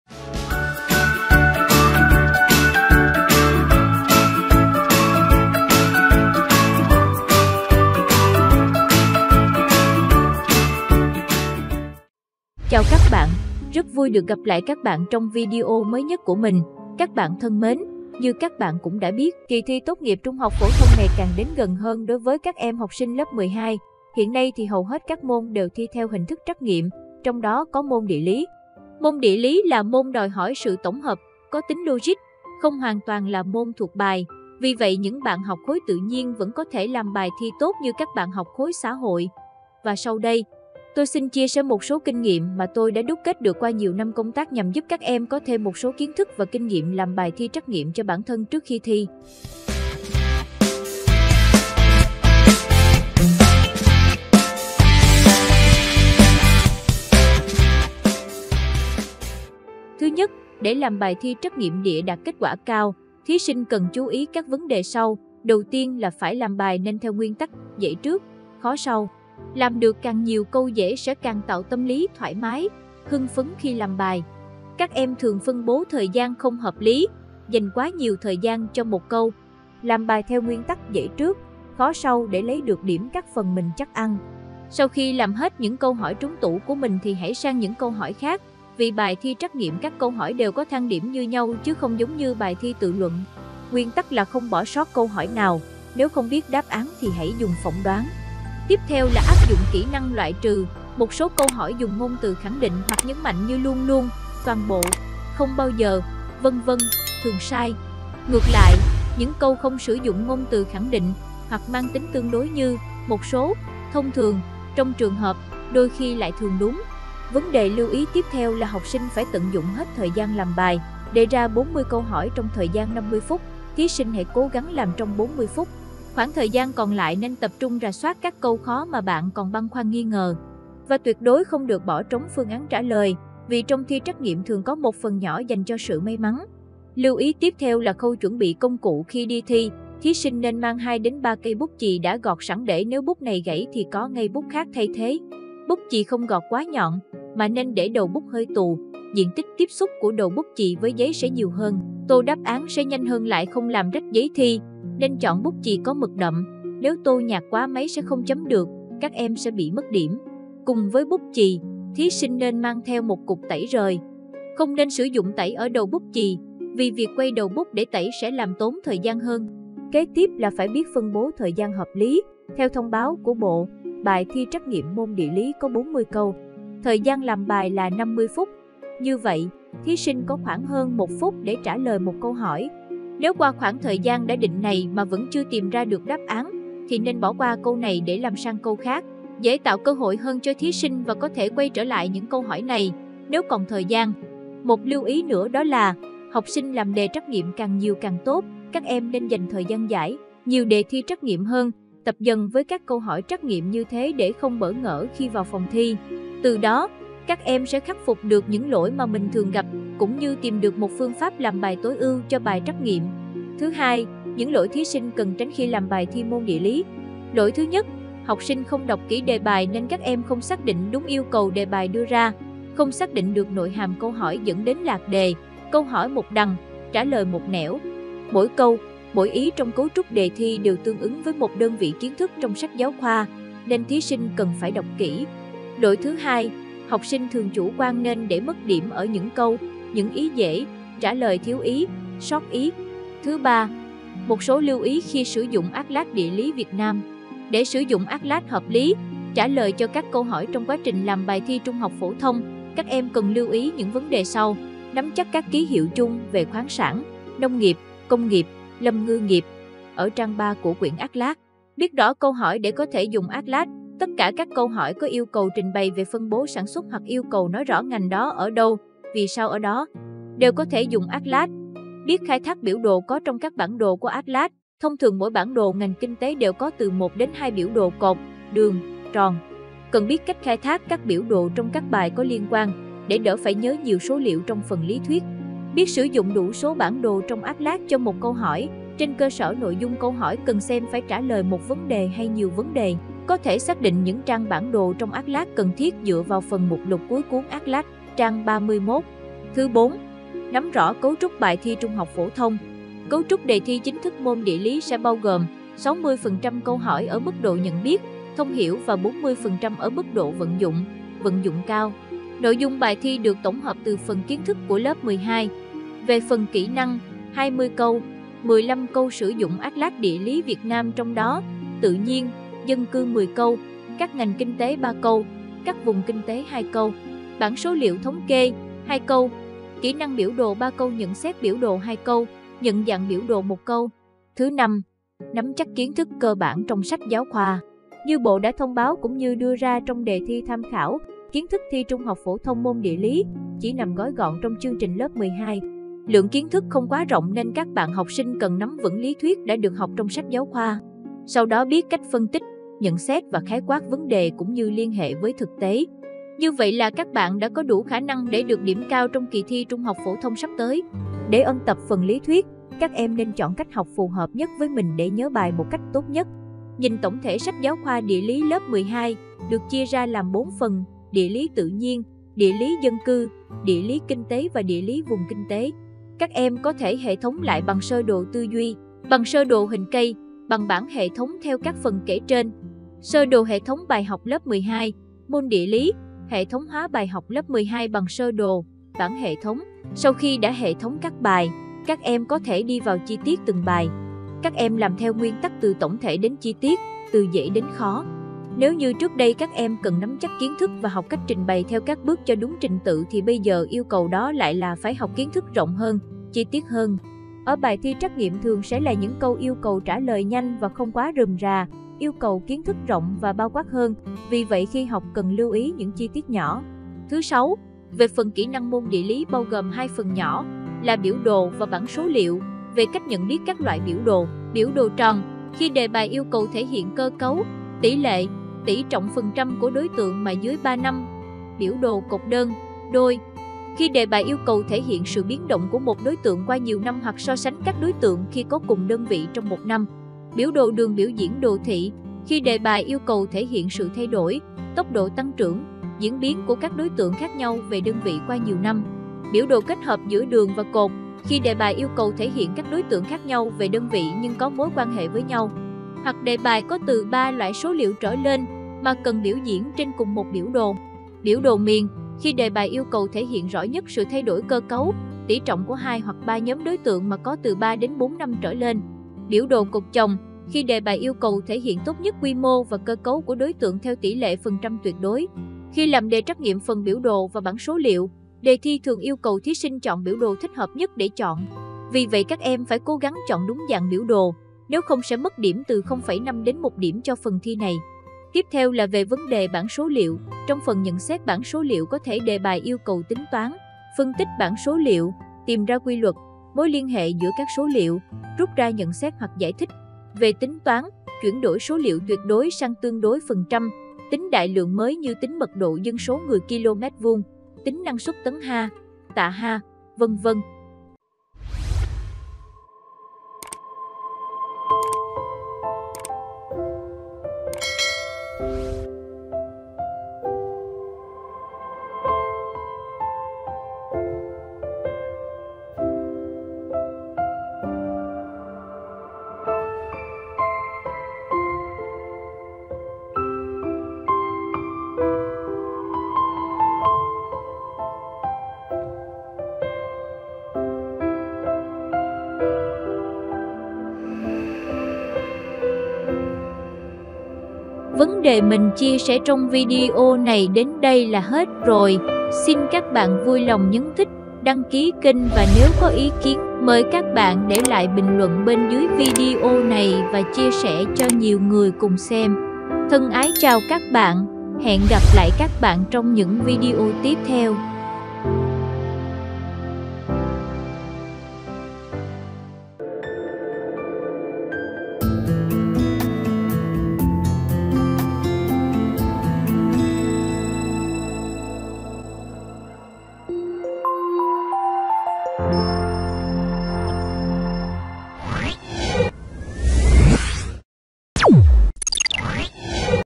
KINH NGHIỆM THI TRẮC NGHIỆM MÔN ĐỊA LÍ 12 ĐẠT KẾT QUẢ CAO🌟✍🏞
Thứ nhất:  Đầu tiên là phải Làm bài nên theo nguyên tắc: “Dễ trước, khó sau”. Tiếp theo là áp dụng Kỹ năng loại trừ:  Một số câu hỏi dùng ngôn từ khẳng định hoặc nhấn mạnh như “luôn luôn”, “toàn bộ”, “không bao giờ”, vân vân, thường sai. Ngược lại, những câu không sử dụng ngôn từ khẳng định, hoặc mang tính tương đối như: “một số”, “thông thường”, “trong trường hợp”, “đôi khi” lại thường đúng. Vấn đề lưu ý tiếp theo là học sinh phải Tận dụng hết thời gian làm bài: Đề ra 40 câu hỏi trong thời gian 50 phút, Thí Sinh hãy cố gắng làm trong 40 phút. Khoảng thời gian còn lại nên tập trung rà soát các câu khó mà bạn còn băn khoăn nghi ngờ. Và tuyệt đối không được bỏ trống phương án trả lời: Tiếp theo là khâu chuẩn bị Công cụ khi đi thi:  Thí Sinh nên mang 2 đến 3 cây bút chì đã gọt sẵn để nếu bút này gãy thì có ngay bút khác thay thế.  Phân bố thời gian hợp lý: Theo thông báo của Bộ, bài thi trắc nghiệm môn địa lí có 40 câu. Thời gian làm bài là 50 phút.  Làm đề trắc nghiệm càng nhiều càng tốt: các em nên dành thời gian giải nhiều đề thi trắc nghiệm hơn, tập dần với các câu hỏi trắc nghiệm như thế để không bỡ ngỡ khi vào phòng thi. Từ đó, các em sẽ khắc phục được những lỗi mà mình thường gặp, cũng như tìm được một phương pháp làm bài tối ưu cho bài trắc nghiệm.
 Thứ hai: Những lỗi thí sinh cần tránh khi làm bài thi môn Địa lí.
Lỗi thứ nhất, học sinh không đọc kỹ đề bài nên các em không xác định đúng yêu cầu đề bài đưa ra, không xác định được nội hàm câu hỏi dẫn đến lạc đề, câu hỏi một đằng, trả lời một nẻo. Mỗi câu, mỗi ý trong cấu trúc đề thi đều tương ứng với một đơn vị kiến thức trong sách giáo khoa, nên thí sinh cần phải đọc kỹ.
Lỗi thứ 2, học sinh thường chủ quan nên để mất điểm ở những câu, những ý dễ, trả lời thiếu ý, sót ý.
Thứ 3. Một số lưu ý khi sử dụng Atlat Địa lí Việt Nam.  Nắm chắc các ký hiệu: ... ở trang 3 của quyển Atlat.  Biết rõ câu hỏi để có thể dùng Atlat: tất cả các câu hỏi có yêu cầu trình bày về phân bố sản xuất hoặc yêu cầu nói rõ ngành đó ở đâu, vì sao ở đó… đều có thể dùng Atlat.  Biết khai thác biểu đồ có trong các bản đồ của Atlat: thông thường mỗi bản đồ ngành kinh tế đều có từ 1 đến 2 biểu đồ (cột, đường, tròn...), cần biết cách khai thác các biểu đồ trong các bài có liên quan, để đỡ phải nhớ nhiều số liệu trong phần lý thuyết.  Biết sử dụng đủ số bản đồ trong Atlat cho một câu hỏi: trên cơ sở nội dung câu hỏi cần xem phải trả lời một vấn đề hay nhiều vấn đề, có thể xác định những trang bản đồ trong Atlat cần thiết dựa vào phần mục lục cuối cuốn Atlat, (trang 31).
Thứ 4. Nắm rõ cấu trúc bài thi trung học phổ thông.
Cấu trúc đề thi chính thức môn Địa lý sẽ bao gồm: 60% câu hỏi ở mức độ nhận biết, thông hiểu và 40% ở mức độ vận dụng, vận dụng cao.
Thứ 5. Nắm chắc kiến thức cơ bản trong sách giáo khoa.
Thứ sáu, về phần kỹ năng môn Địa lý:
 Về cách nhận biết Các loại biểu đồ:
 Biểu đồ tròn: khi đề bài yêu cầu thể hiện cơ cấu, tỉ lệ, tỉ trọng (phần trăm) của đối tượng mà dưới 3 năm.
 Biểu đồ cột (đơn, đôi…): khi đề bài yêu cầu thể hiện sự biến động của một đối tượng qua nhiều năm hoặc so sánh các đối tượng khi có cùng đơn vị trong một năm.
 Biểu đồ đường biểu diễn (đồ thị): khi đề bài yêu cầu thể hiện sự thay đổi, tốc độ tăng trưởng, diễn biến của các đối tượng khác nhau về đơn vị qua nhiều năm.
Biểu đồ kết hợp giữa đường và cột: khi đề bài yêu cầu thể hiện các đối tượng khác nhau về đơn vị nhưng có mối quan hệ với nhau. Hoặc đề bài có từ ba loại số liệu trở lên, mà cần biểu diễn trên cùng một biểu đồ.
Biểu đồ miền: khi đề bài yêu cầu thể hiện rõ nhất sự thay đổi cơ cấu, tỉ trọng của hai hoặc ba nhóm đối tượng mà có từ 3 đến 4 năm trở lên…
Biểu đồ cột chồng: khi đề bài yêu cầu thể hiện tốt nhất quy mô và cơ cấu của đối tượng (theo tỉ lệ phần trăm tuyệt đối).
Bảng số liệu: trong phần nhận xét bảng số liệu có thể đề bài yêu cầu tính toán, phân tích bảng số liệu, tìm ra quy luật, mối liên hệ giữa các số liệu, rút ra nhận xét hoặc giải thích. Về tính toán: Chuyển đổi số liệu tuyệt đối sang tương đối (%); tính đại lượng mới như tính mật độ dân số (người/km2); tính năng suất (tấn/ha; tạ/ha), ….